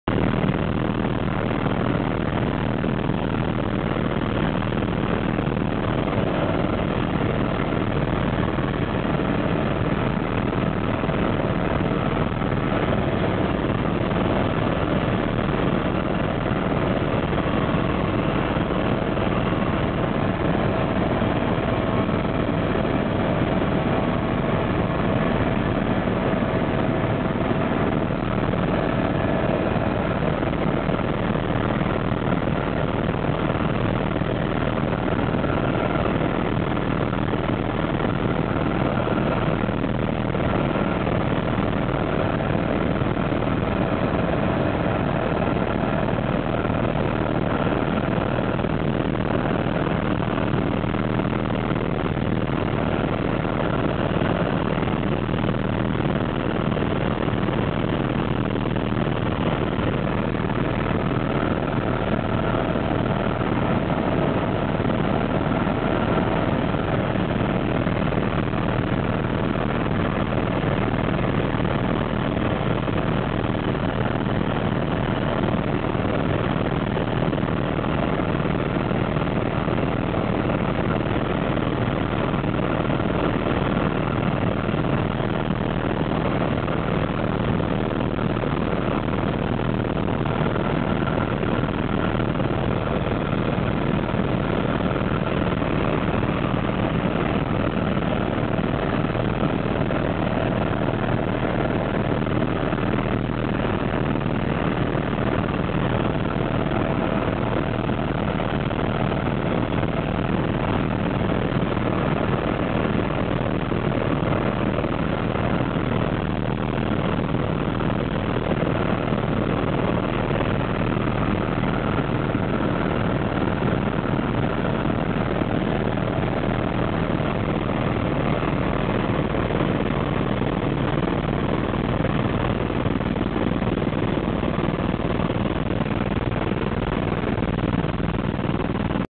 Helicopter Tour Las Vegas Strip 2011 Maverick
Return trip from Grand Canyon over the Las Vegas Strip